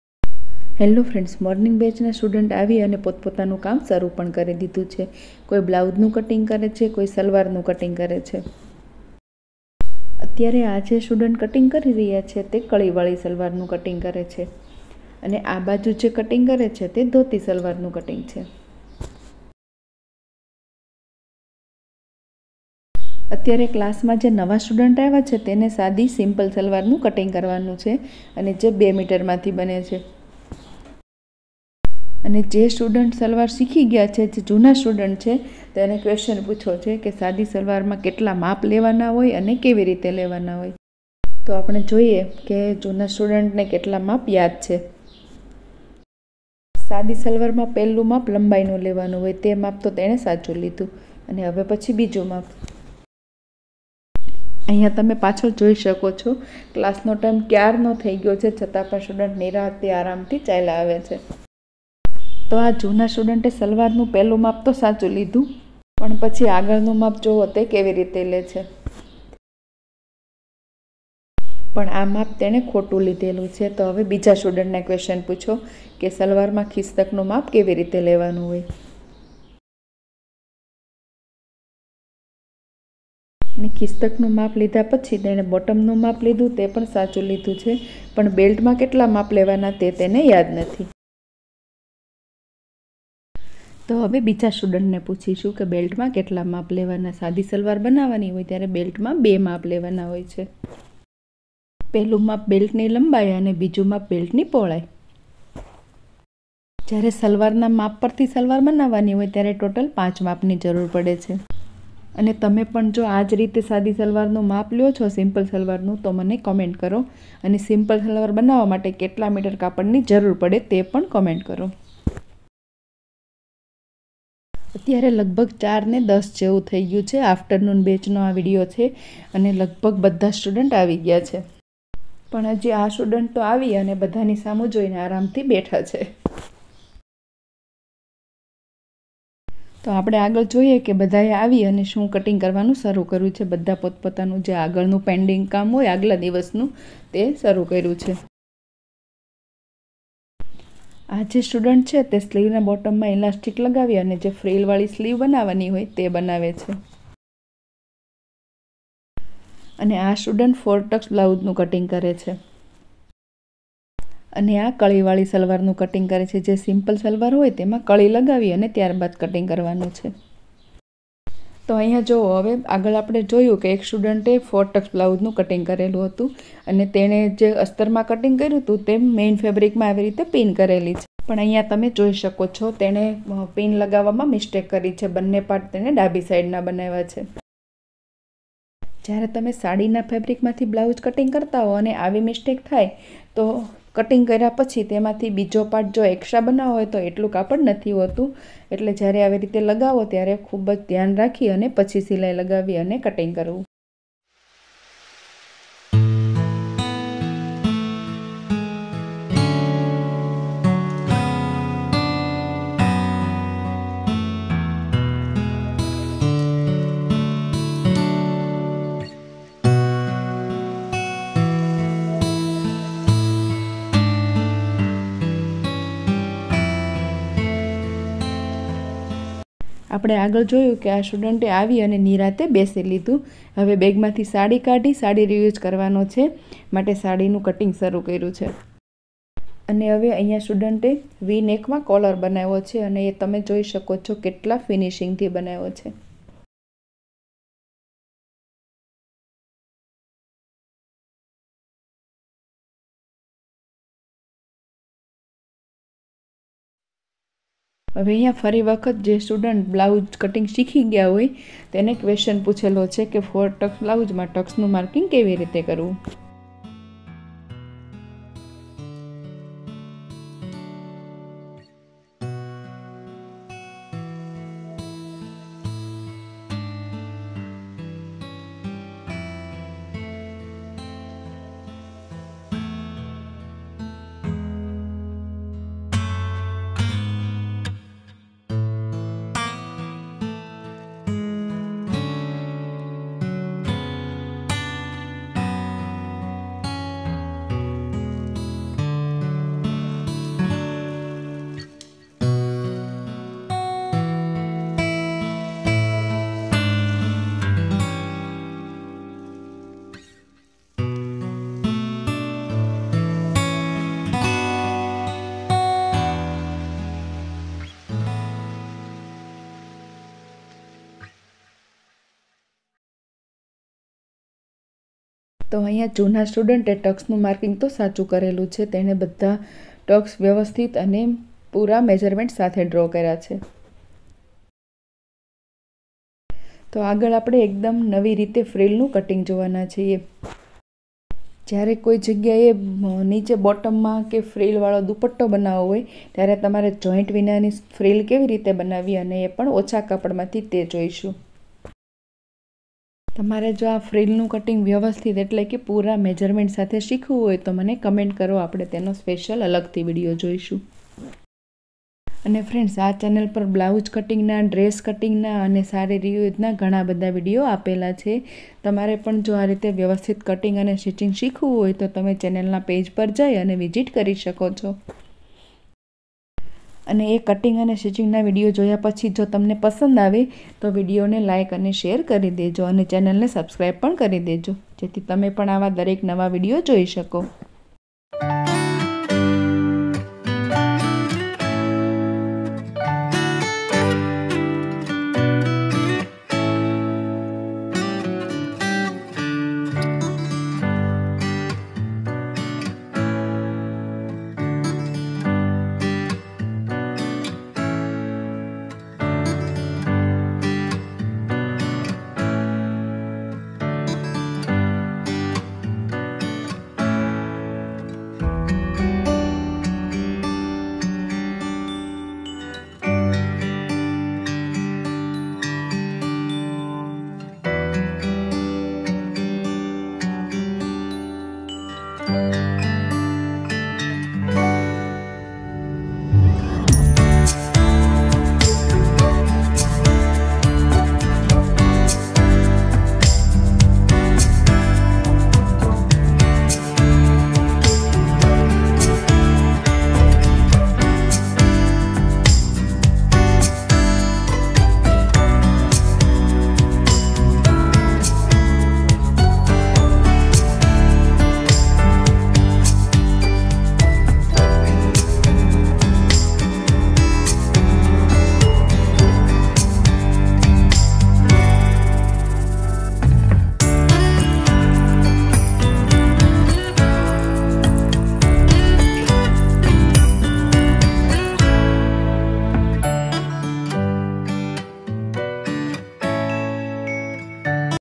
cutting and stitching✂️ silai class live👍Blouse cutting✂ salwar cutting
first vlog 👍

about👉આ વીડિયો મા સિલાઈ કલાસ મા સ્ટુડન્ટ કેવી રીતે કામ કરે છે તે બતાવવા મા આવ્યુ છે🤷‍♀️👍

your queries👇
collar cutting
kurti cutting
salwar cutting
kurti fitting
ready made kurti fitting
ready made kurti